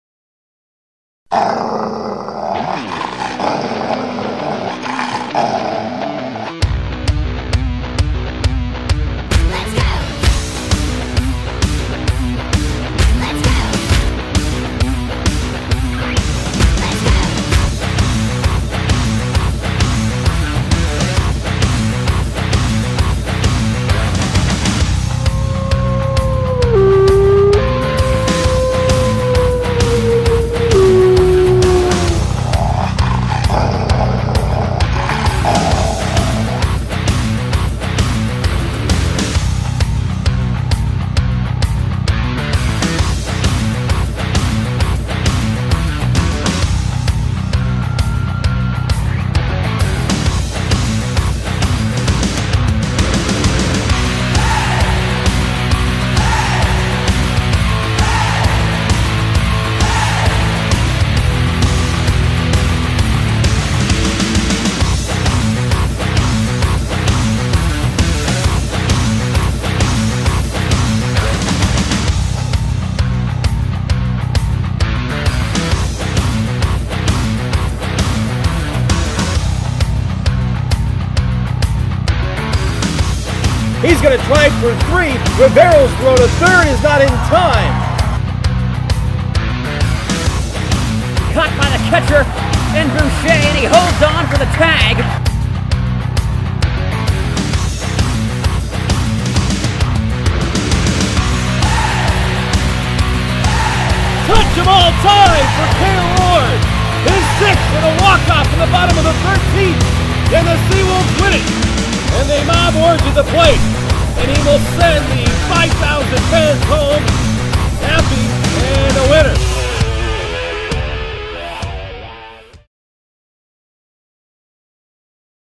Erie SeaWolves comic book video
Produced as pre-game, team introduction video and for in-game, inning breaks. Video highlights SeaWolves players, in-game entertainment and fans in a unique, comic-book style.